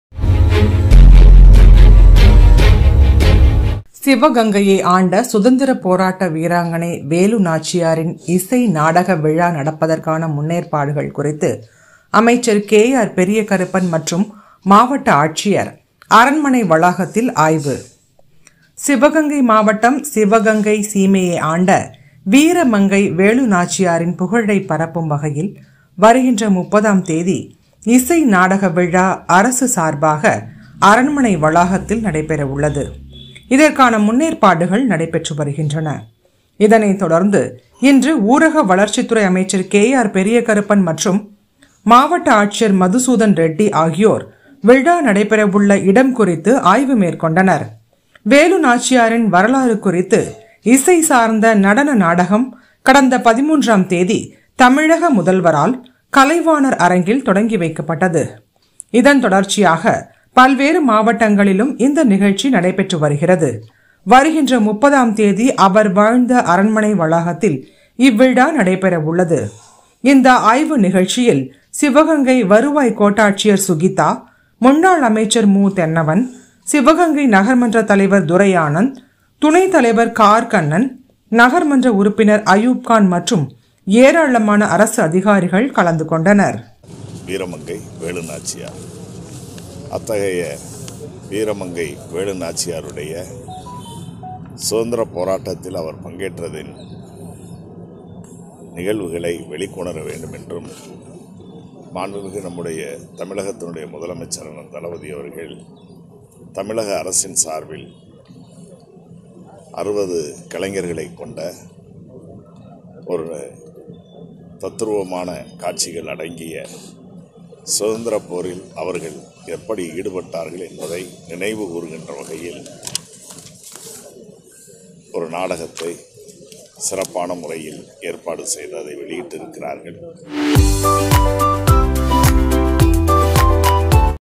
சிவகங்கையை ஆண்ட சுதந்திர போரட்ட வீராங்கனை வேலு நாச்சியாரின் இசைநாடக விழா...
சிவகங்கையை ஆண்ட சுதந்திர போரட்ட வீராங்கனை வேலு நாச்சியாரின் இசைநாடக விழா நடப்பதற்கான முன்னேற்பாடுகள் குறித்து அமைச்சர் கே ஆர் பெரிய கருப்பன் மற்றும் மாவட்ட ஆட்சியர் அரண்மனை வளாகத்தில் ஆய்வு...